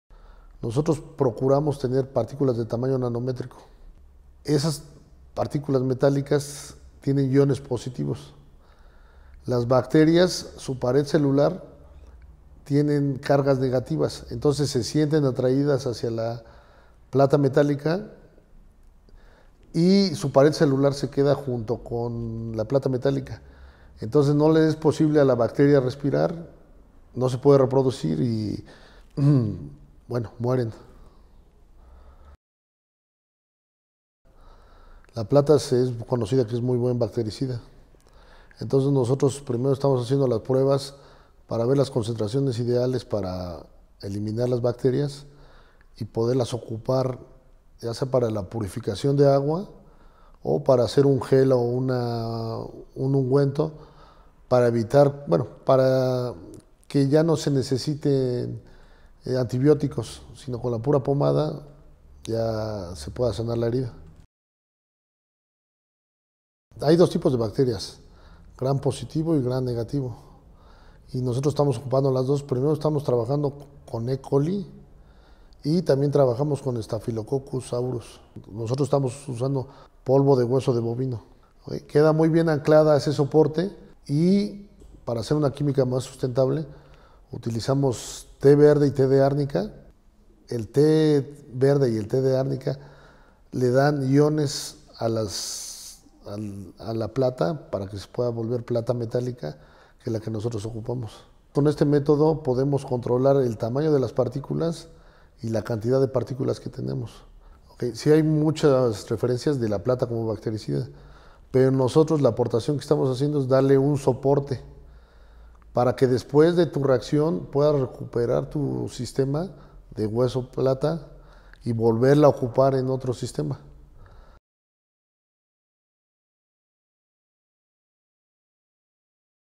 Nanopartículas de Plata
El Dr. Sergio Gama, académico e investigador de la Universidad del Valle de México Campus Toluca, desarrolló un método sustentable y biodegradable para matar bacterias, a partir de nanopartículas de plata en soporte de hueso de bovino y sintetizadas con té de árnica y té verde. Las nanopartículas podrían ser utilizadas parala purificación de agua y tratamiento de heridas.